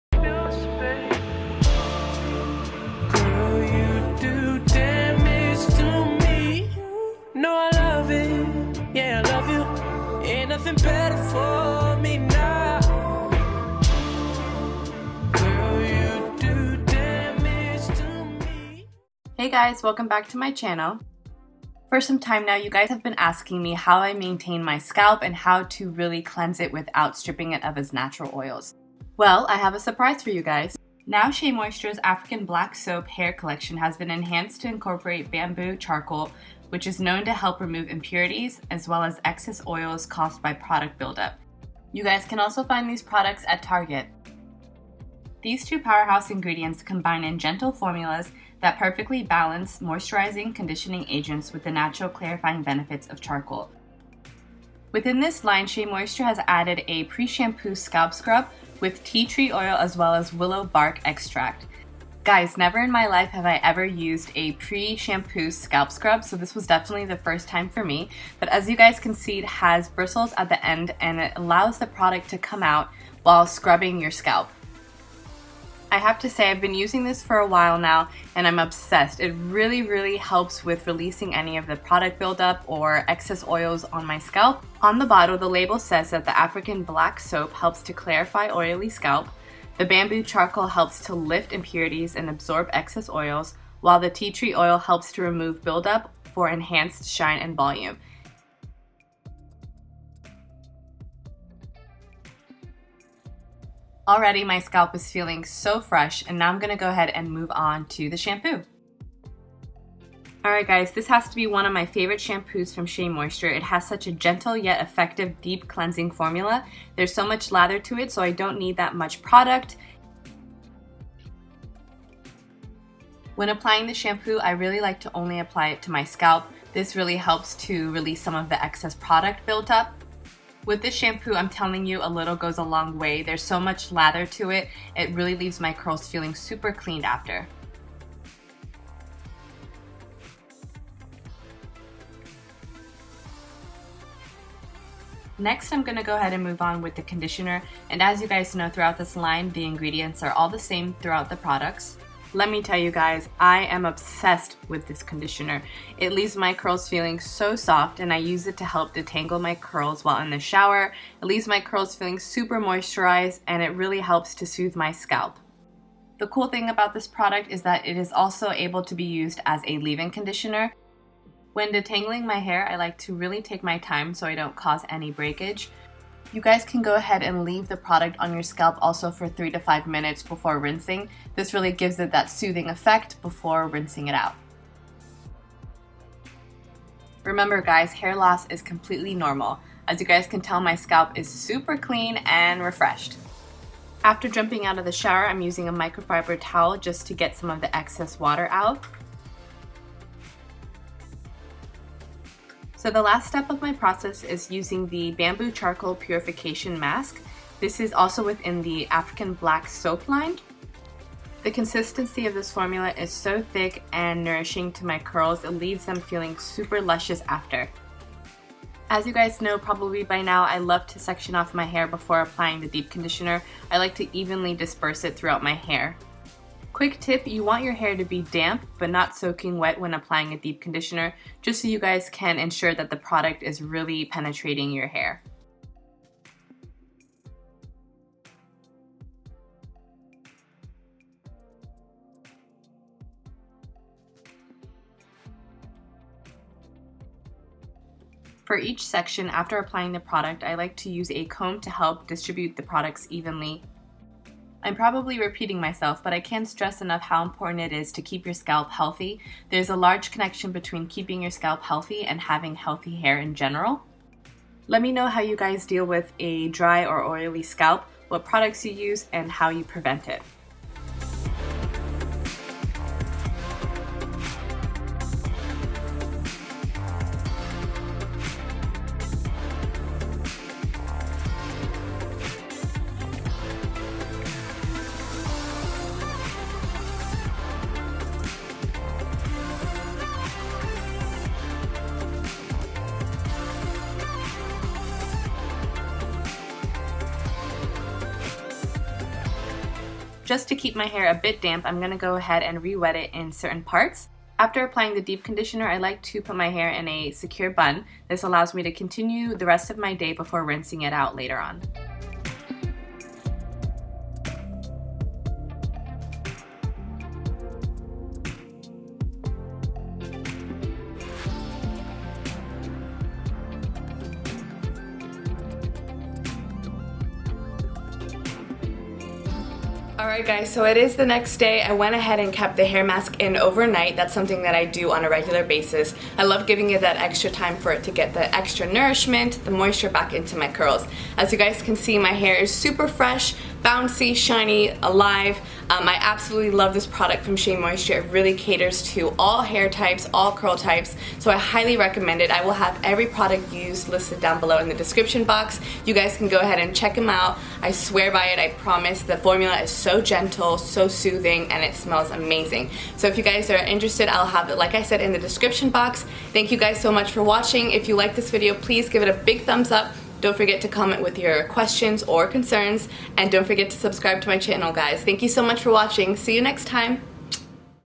Easy Steps To Maintain A Healthy Scalp
Hey guys welcome back! In today's video I will be sharing my favorite products that I use to help maintain a healthy scalp. The relationship between a healthy scalp and having healthy hair are so important for so many reasons. I hope you all enjoy! Xoxo